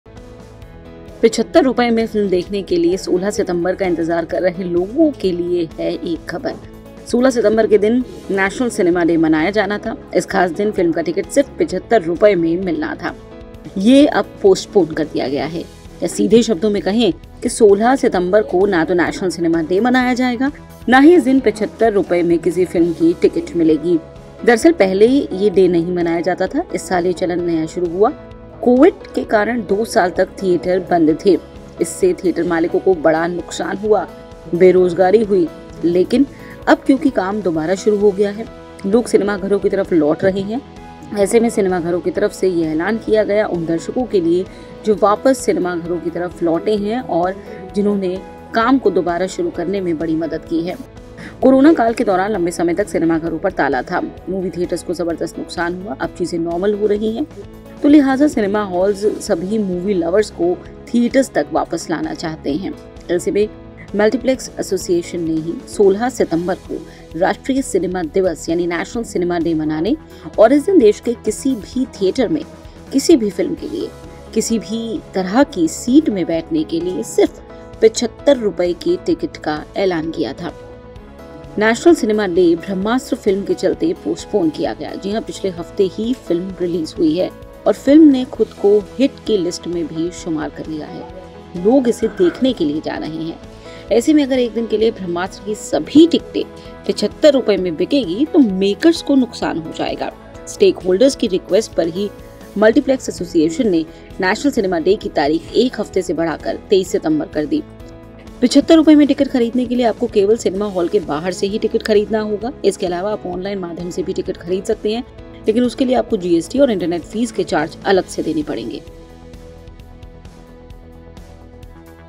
सिनेमा डे को क्यों अब किसी और दिन मनाया जाएगा. National cinema day postponed| entertainment news
फिल्म देखने के लिए 16 सितंबर का इंतज़ार कर रहे लोगों के लिए बुरी खबर है, जिस 16 सितम्बर के दिन नेशनल सिनेमा डे (National Cinema Day) मनाया जाना था और इस खास दिन फिल्म की टिकट सिर्फ 75 रुपए में मिलनी थी, वो कार्यक्रम पोस्टपोंड कर दिया गया है. सीधे शब्दों में कहें तो अब 16 सितम्बर को ना तो नेशनल सिनेमा डे मनाया जाएगा ना सस्ती टिकट मिलेगी.

There is bad news for the people waiting for 16 September to watch the film, on which National Cinema Day was to be celebrated and on this special day, the ticket for the film was to be got for only Rs 75, that program was postponed. Has been done. Simply put, now on September 16, neither National Cinema Day will be celebrated nor will you get a movie ticket for 75 rupees on this day.एगा और ना ही इस दिन 75 रुपए में किसी फिल्म की टिकट मिलेगी..